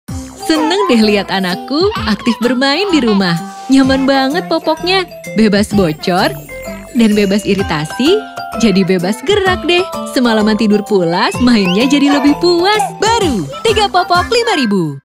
Baru Kemasan Renceng Genki Moko Moko
Halo Ma, kini hadir kemasan BARU:

Popok Genki Moko Moko Renceng Isi 3 harga Rp 5.000, Pertama di Indonesia!

Genki Moko Moko siap menemani si Kecil beraktivitas dimana aja dan kapan aja dengan TripleBebas.

1. Bebas Bocor 🙅🏻‍♀️💧
Dibuat dengan teknologi Maxi Gel yang berdaya serap tinggi, Mama dapat mendukung aktivitas si Kecil sepanjang hari.

2. Bebas Iritasi 👶🏻
Terbuat dari bahan selembut kapas dan permukaan berpori, si Kecil dapat bebas bermain dengan nyaman tanpa cemas iritasi.

3. Bebas Gerak ⚽️🏃🏻‍♂️
Desain popok tipis dan karet pinggang elastis memastikan popok mengikuti lekuk tubuh si Kecil sehingga ia dapat bebas bergerak.

Yuk Ma dukung si Kecil BebasAktif setiap hari. Beli Genki Moko Moko sekarang di toko-toko terdekat!